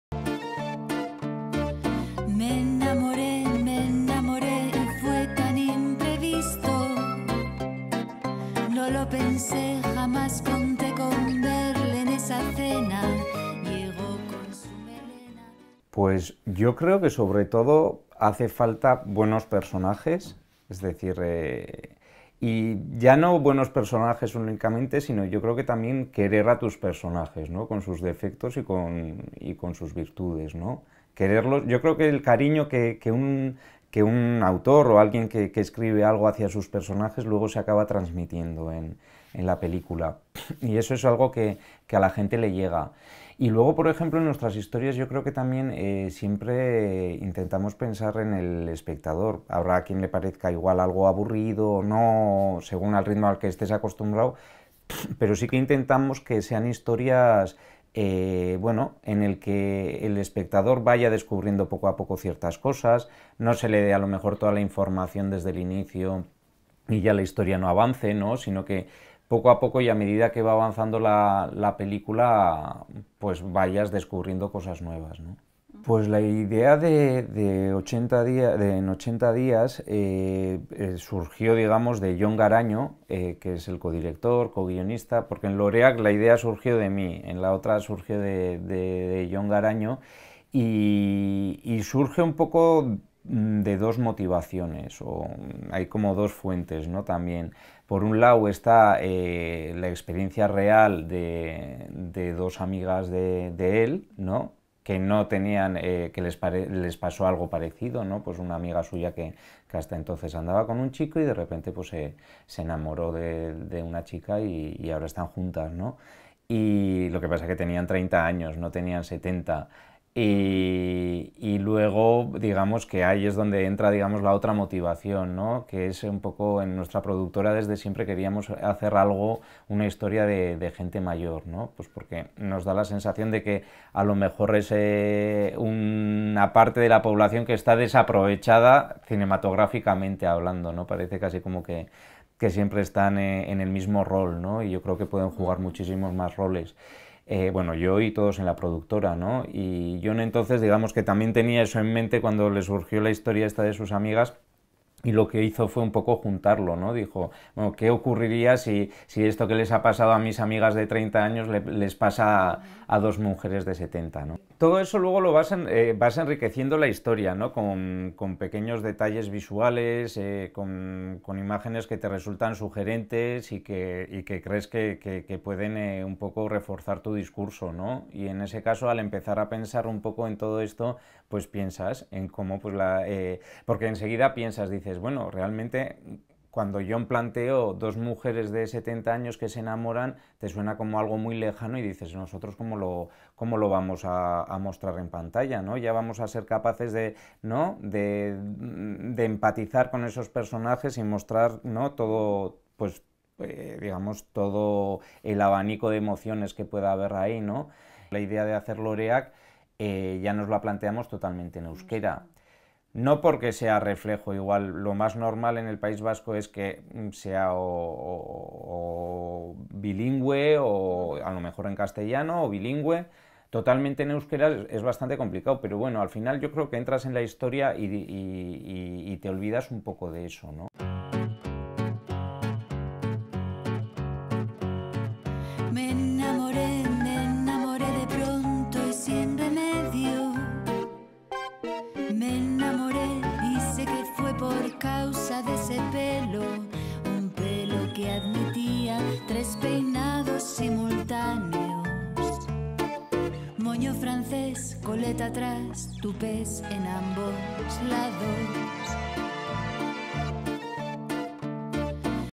Interview with José Mari Goenaga, director of "Flowers" and 80 days"
Edinburgh Spanish Film Festival, 2nd to 10th October 2015, Filmhouse, Edinburgh
Music by Single
Camera: Ivan Umpiérrez
Edition: Cristina Liébana
Interviewer: Marian A. Aréchaga
Transcription: Alicia López
Translation: Ramiro Bestilleiro Rey
GFX: Laura Mures
Production: Guillermo Carnero Rosell.